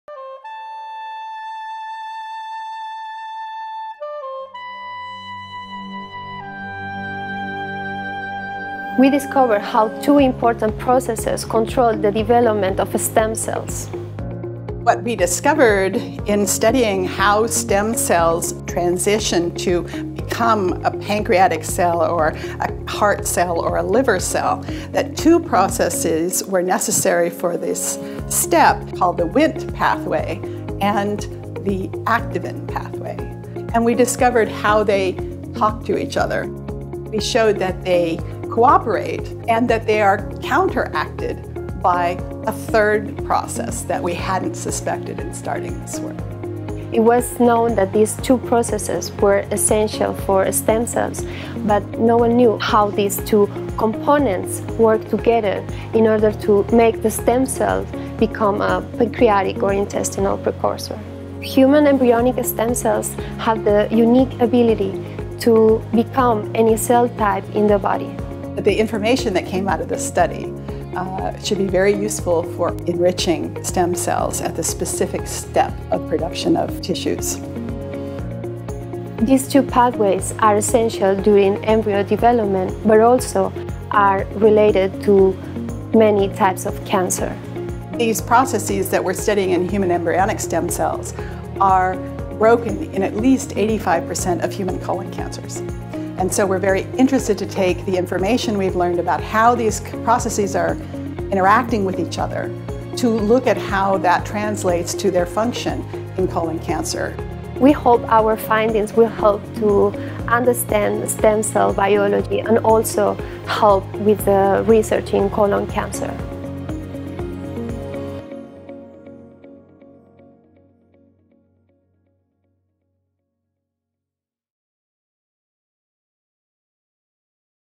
New Stem Cell Findings Help to Understand Colon Cancer
Findings in the laboratory of Katherine Jones at The Salk Institute in La Jolla, CA could aid regenerative and cancer therapies.